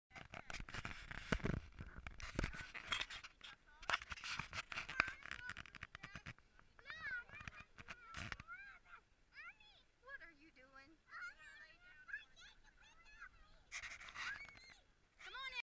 Sand Angels
Kids making sand angels at the beach